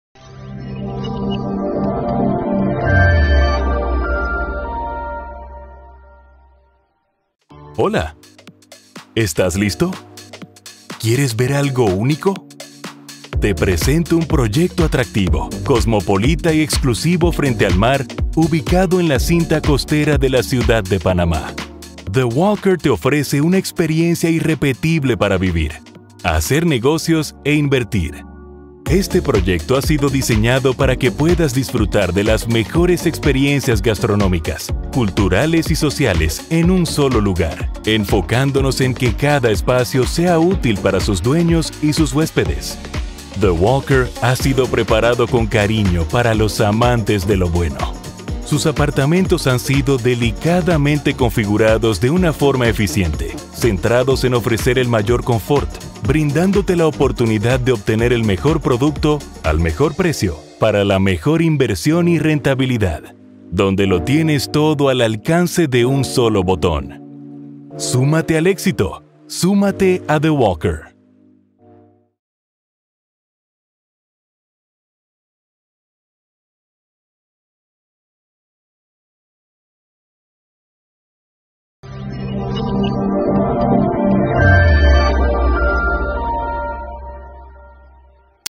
🌊 Exclusivo proyecto frente al Mar. Conoce THE WALKER by Strohm Teka - Avenida Balboa💎🏙️
THE WALKER by Strohm Teka, se convertirá en el punto de encuentro preferido de la música, la gastronomía, el arte, la coctelería y la cultura, diseñado para los amantes de la vida y para aquellos que desean vivirla al máximo.

Cuenta con un concepto único donde puedes vivir, invertir y hacer negocios, debido a que la torre está diseñada para que tengas todo a un solo botón de distancia.

Lo inmobiliario, la gastronomía, la coctelería, la cultura y el arte se suman, en un solo lugar, en el mejor proyecto de la Avenida Balboa, en THE WALKER by Strohm Teka.

Súmate al proyecto más cosmopolita y diferente de la Ciudad de Panamá, único en su entorno, súmate a THE WALKER by Strohm Teka.